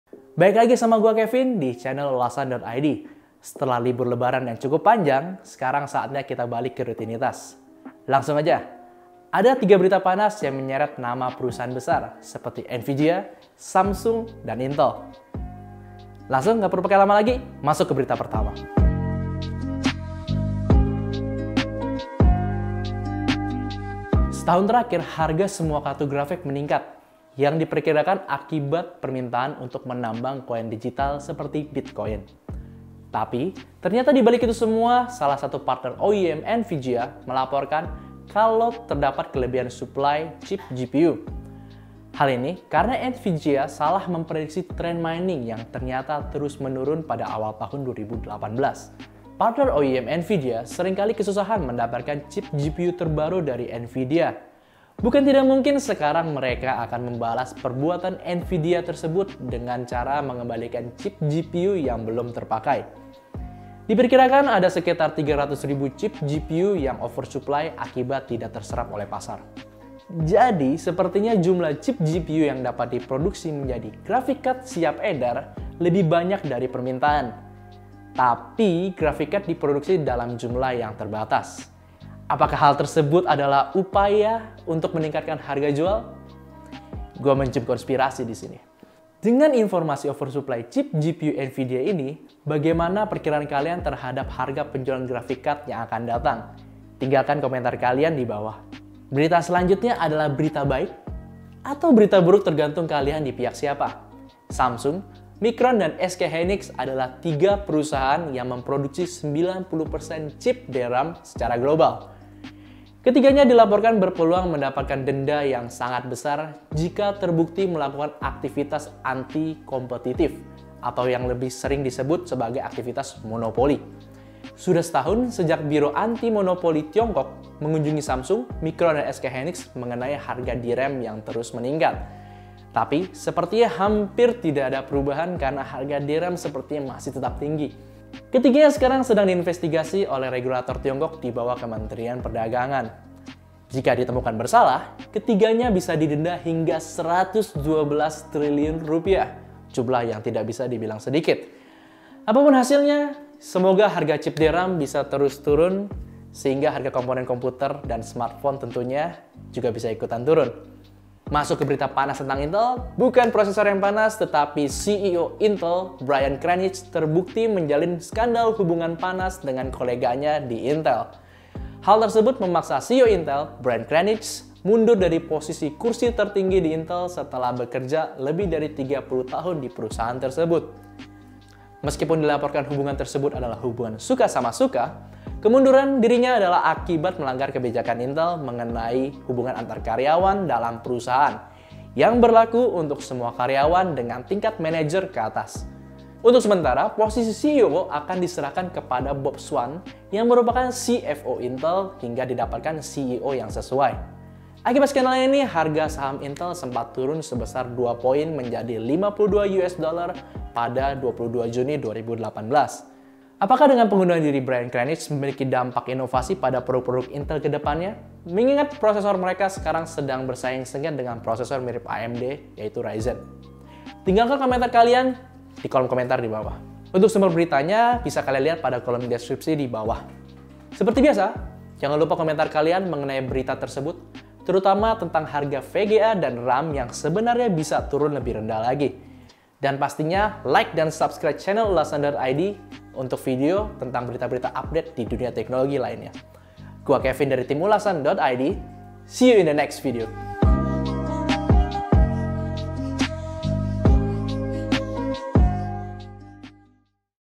GPU Nvidia Over Supply, Samsung akan didenda, Skandal CEO Intel
300.000 CHIP GPU DIKEMBALIKAN KE NVIDIA!

Ulasan ID adalah sebuah channel YouTube dimana kalian dapat menonton, belajar bersama, serta sharing apapun tentang teknologi dan komputer.